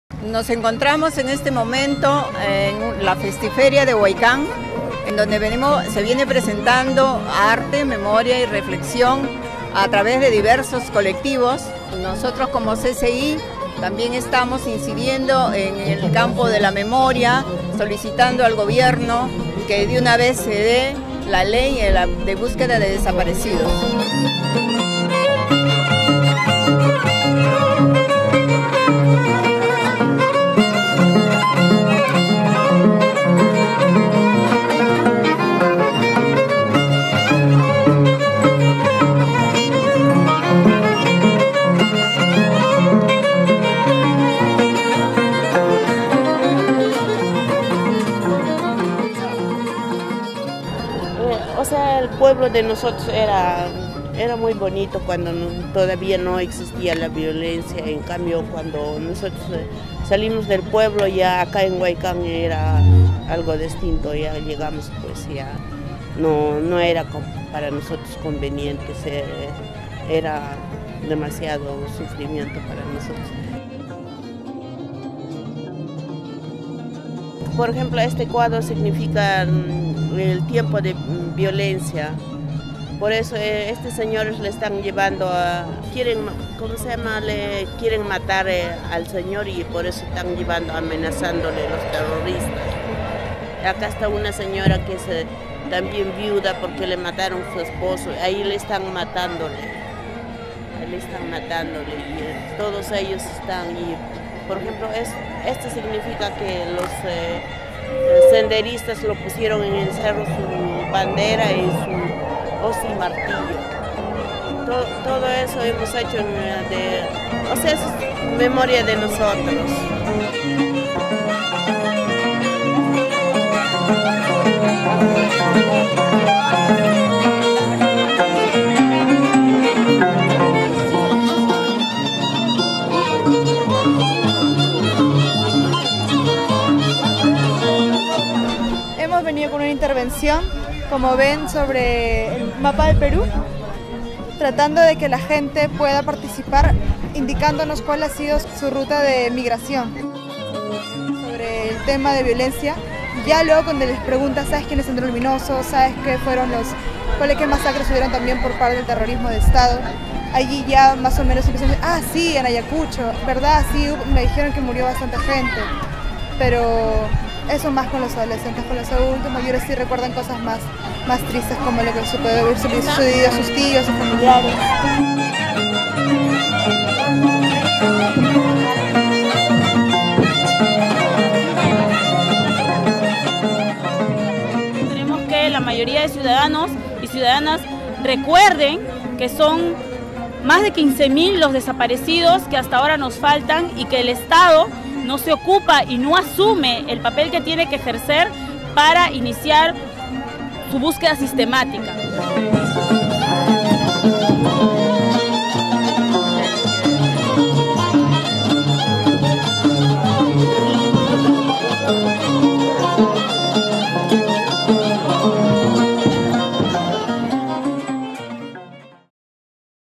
Y de los desaparecid@s ¿Quién habla?
El 30 de agosto es el Día Internacional de los Desaparecidos, quizás sea un día más, un día cualquiera, pero para miles de familias es una fecha en que continúan esperando por años que sus seres queridos, arrebatados una madrugada, mañana o noche, regresen con ellos para ofrecerles una sepultura con dignidad. En el Perú hay más de 15,000 desaparecid@s, levantemos nuestras voces y exijamos al Estado una LEY DE BÚSQUEDA DE LOS DESAPARECIDOS.

Que nos sigan convocando nuestros desaparecidos, que nos hacen falta a tod@s. Ellos ya no están pero viven en todas partes: en nuestra memoria, en nuestros sentimientos, en cada acto de homenaje, en cada palabra levantada en su nombre, en cada esfuerzo, en el cariño de sus padres y hermanos; en el corazón de ustedes, de nosotros.

Lugar: Festi Feria, memoria, arte y reflexión, acción  organizada por LIMAQ en la Plaza de Armas de Huaycán.